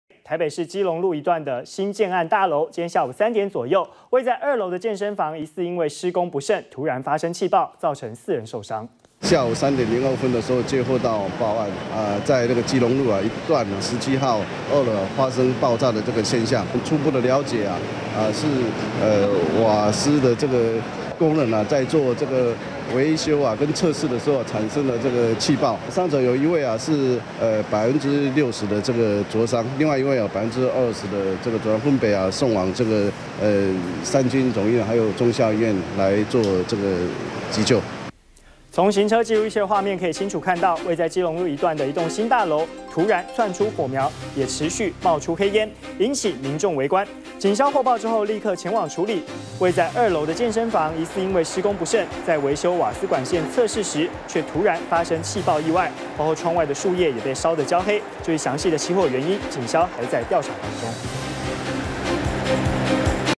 【2014.03.26】新建案大樓施工不慎 瓦斯氣爆4傷 -udn tv
台北市基隆路一段的新建案大樓，今天下午3點左右，位在2樓的健身房疑似因施工不慎，突然發生氣爆，造成四人受傷。完整內容 , FB , Twitter , G+